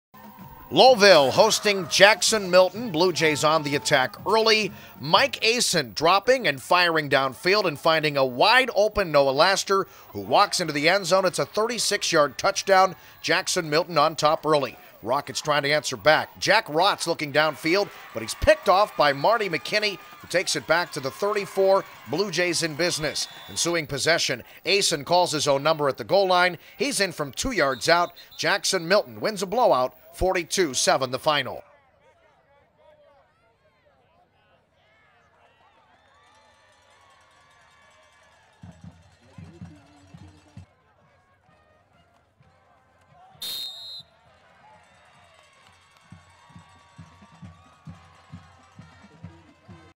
Assion lifts Jackson-Milton; tops Lowellville
Jackson-Milton registered their 6th win of the season to top Lowellville on the road.